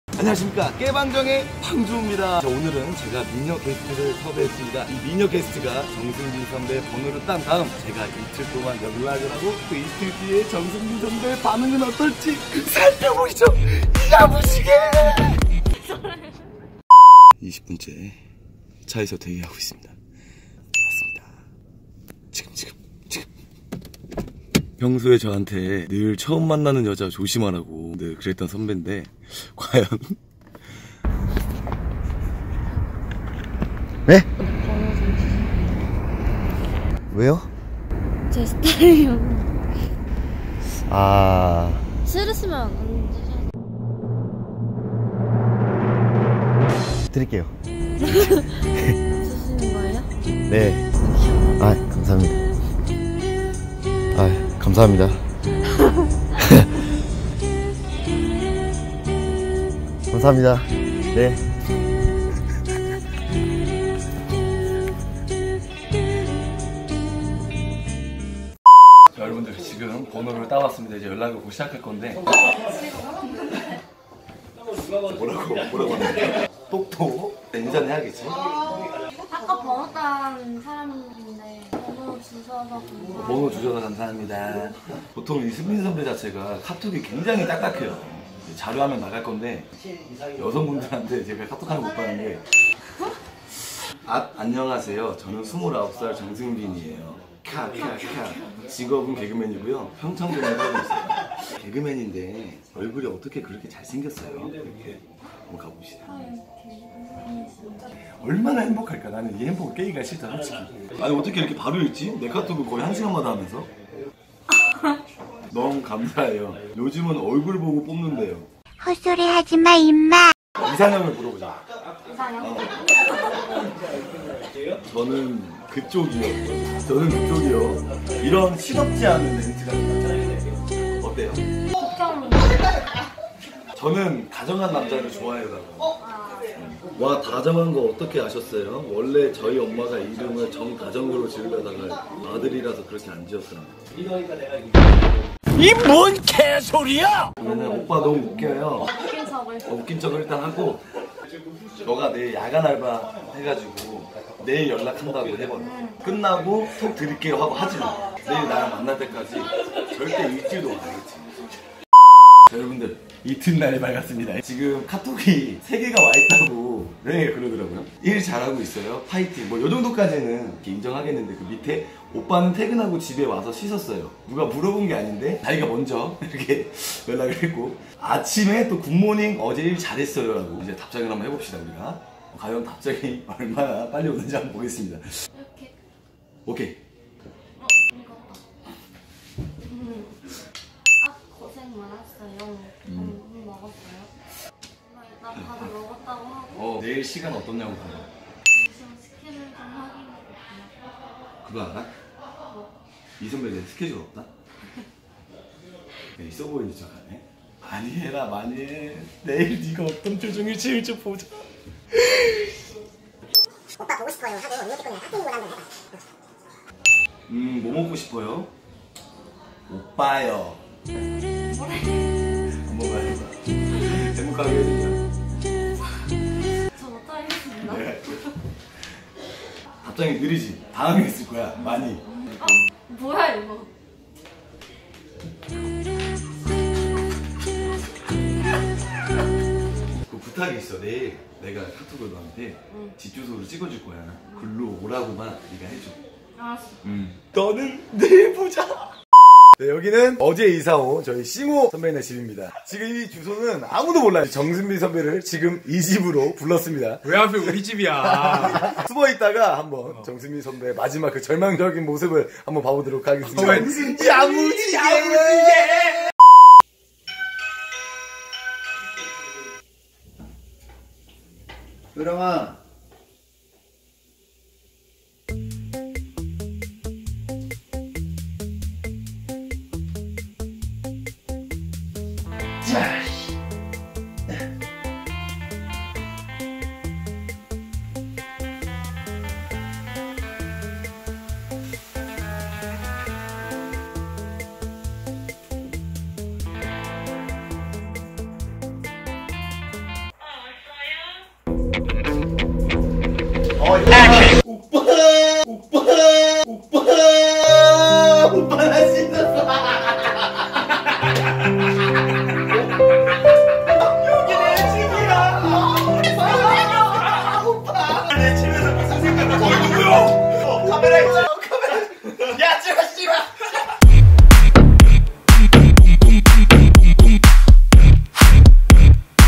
(Sub)몰카)예쁜여자 시켜서 번호따고 3일간 대신연락후 개쪽주깈ㅋㅋㅋㅋ
영상속 여자분(효령) 인스타 -  hyoraeng_
예쁜여자 시켜서 번호받고 3일동안 대신 연락한후 개쪽주깈ㅋㅋㅋ